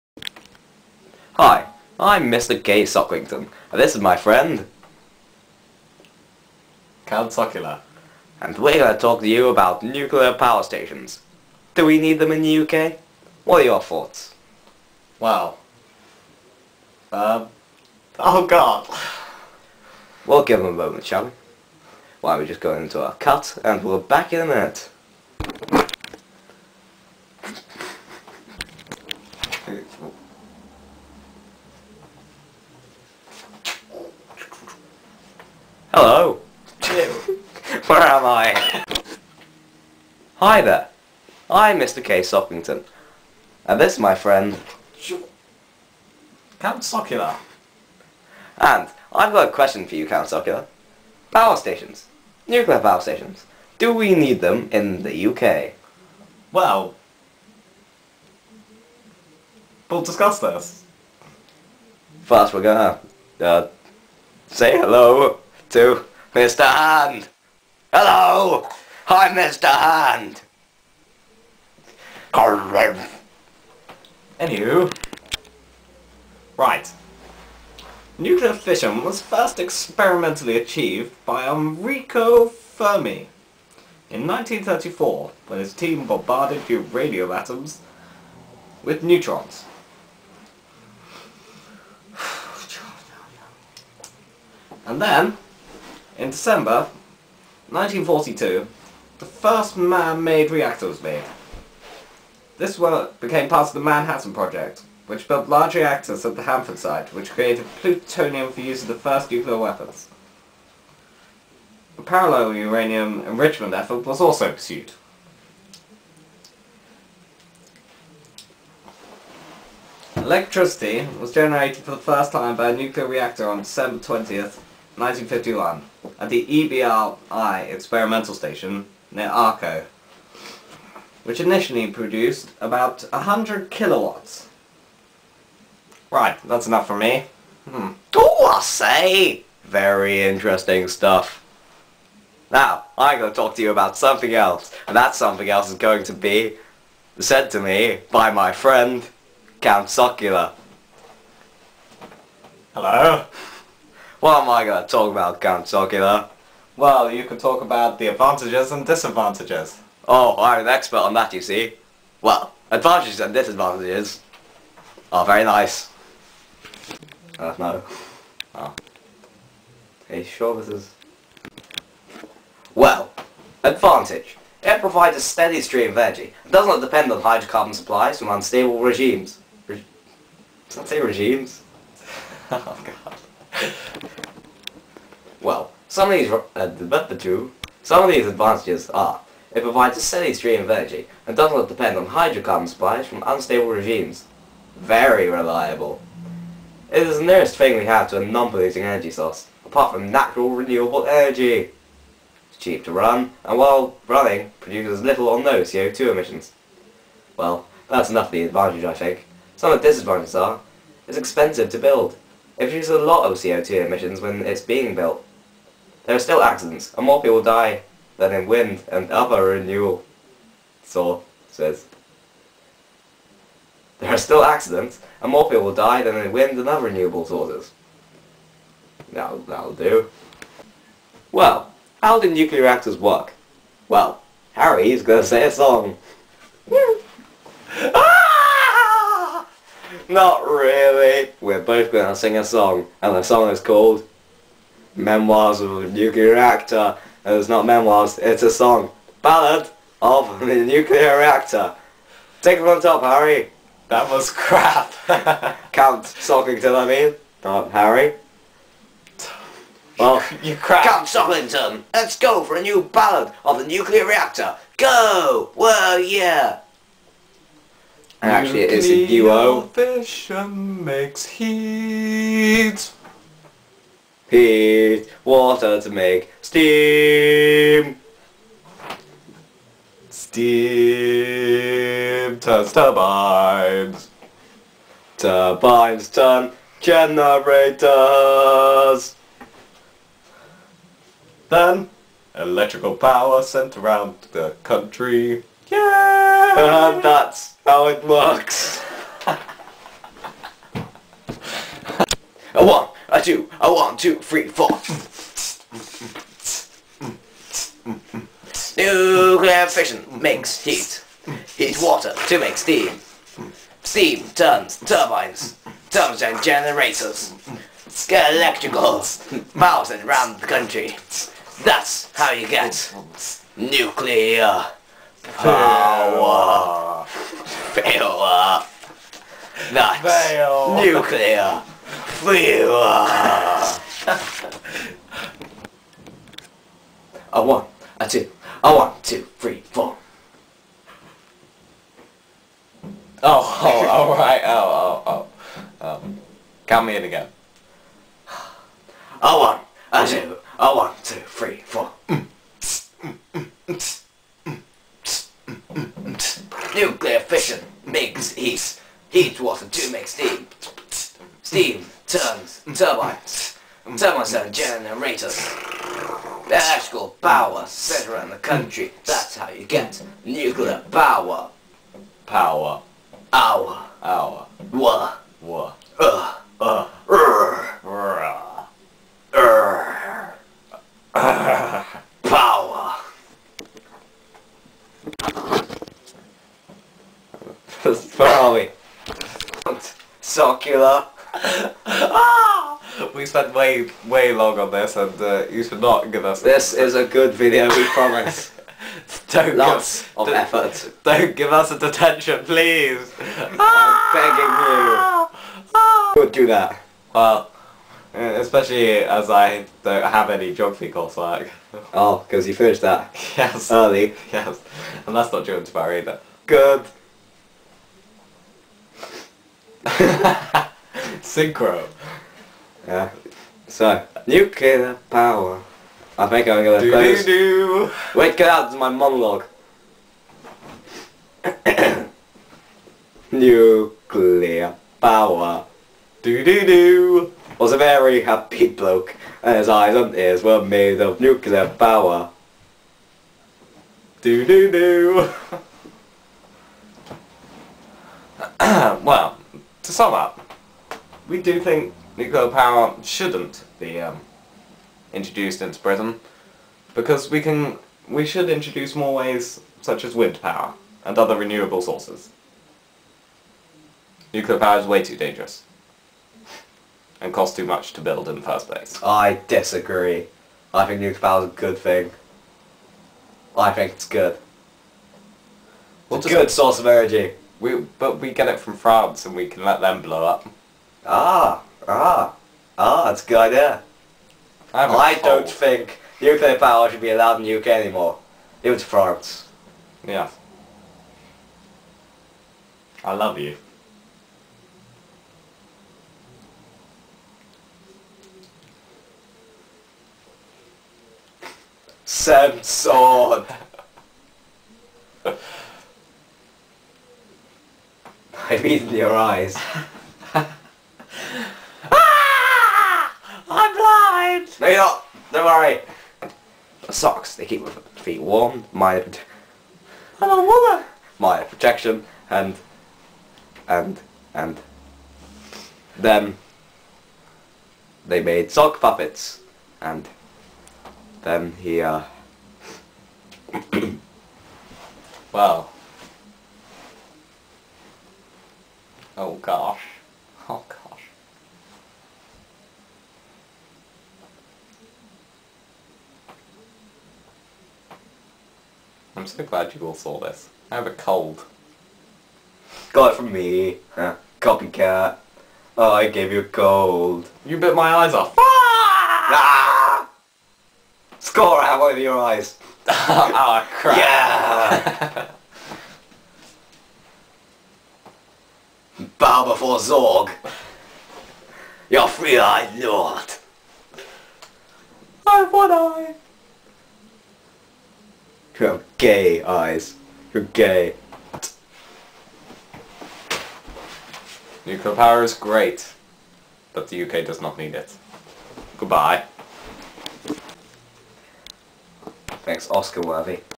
Physics Project
Project Question - Does the UK need new nuclear power stations?
we explain whether the UK does in fact need nuclear power stations. It is a genuine piece of work that we were set over the half term.